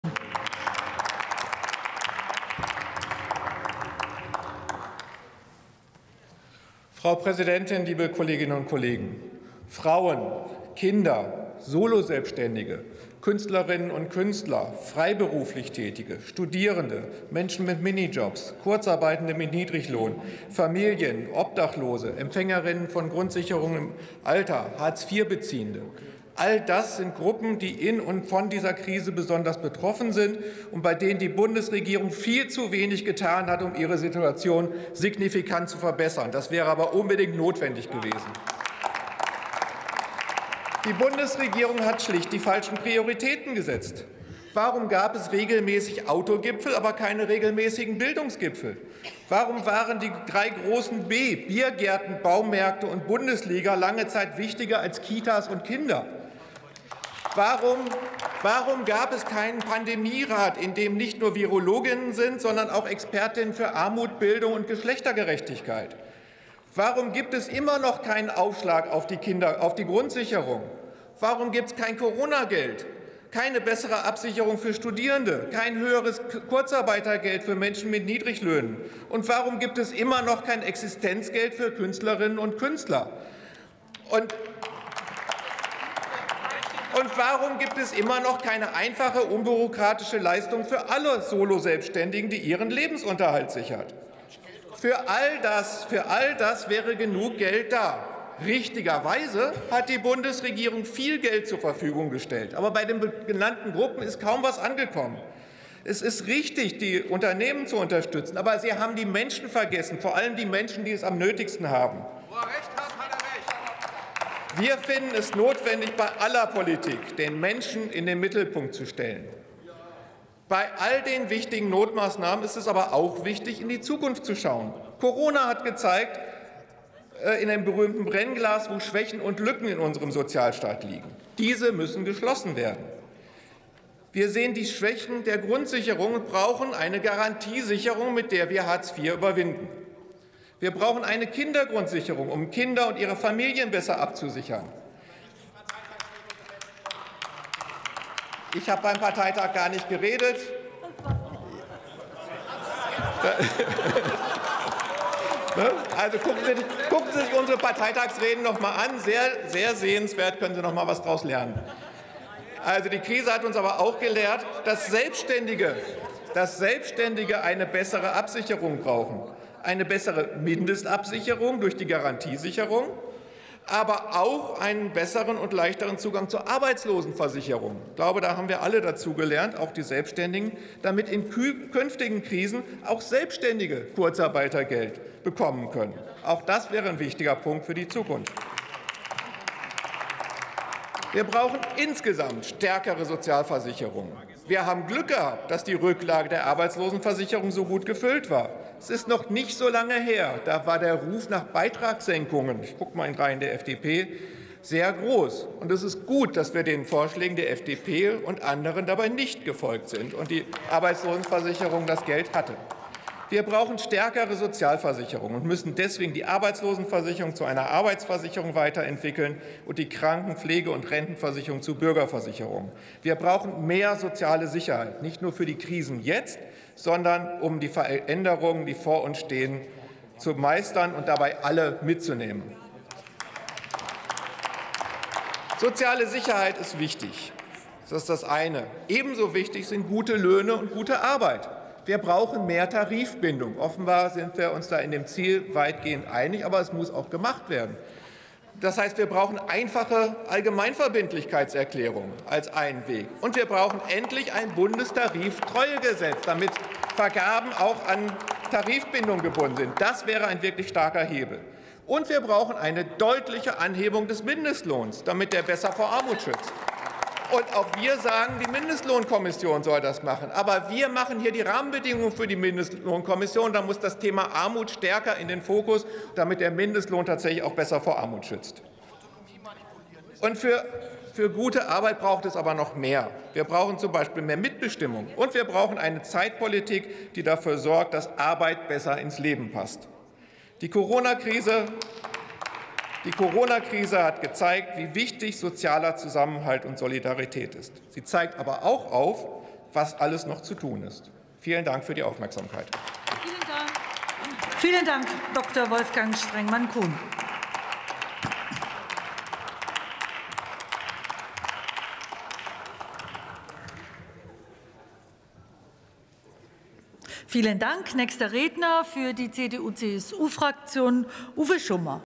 Strengmann-Kuhn, B'90/Grüne: Aktuelle Stunde für gute Löhne und Verteilungsgerechtigkeit sorgen
Warum gibt es immer noch keinen Aufschlag auf die Grundsicherung? Warum gibt es kein Coronageld, keine bessere Absicherung für Studierende, kein höheres Kurzarbeitergeld für Menschen mit Niedriglöhnen? Was ist mit den Soloselbständigen? Und warum gibt es immer noch kein Existenzgeld für Künstlerinnen und Künstler? Es ist richtig, die Unternehmen zu unterstützen. Aber die Bundesregierung hat die Menschen vergessen, vor allem die Menschen, die es am nötigsten haben. Meine Rede zur Aktuellen Stunde in Textform gibt es hier: